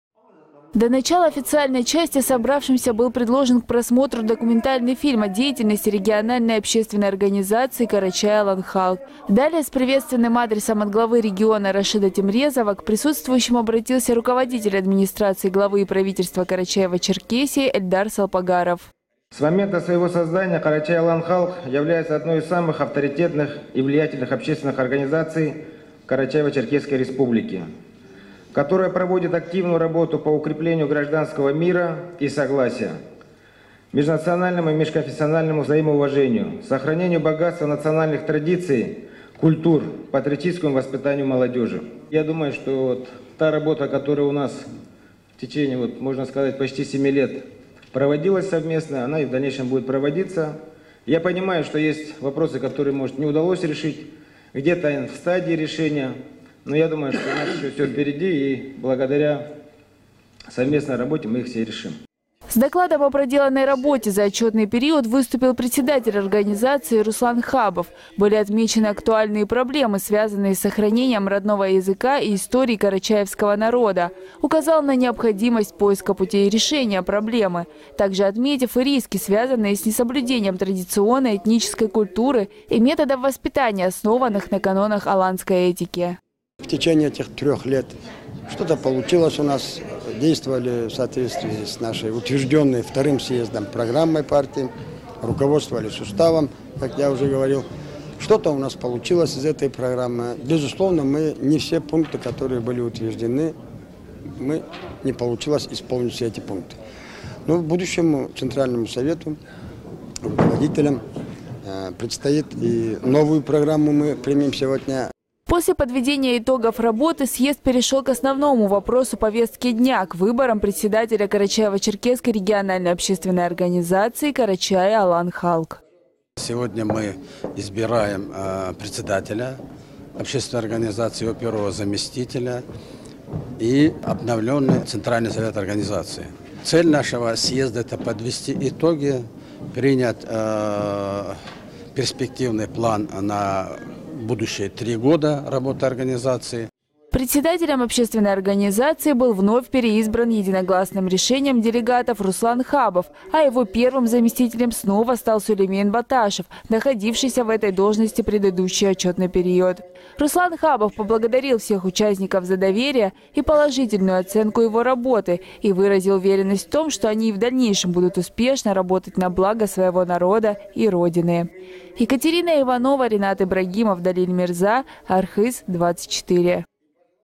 В Черкесске состоялся III съезд "Къарачай Алан Халкъ"
Третий отчетно-выборный съезд Карачаево-Черкесской региональной общественной организации по содействию, защите прав и законных интересов карачаевского народа "Къарачай Алан Халкъ" состоялся в Черкесске. Основные цели встречи - подведение итогов работы за период с 2014 по 2017 годы, выборы в Центральный Совет организации и другие структурные рабочие органы.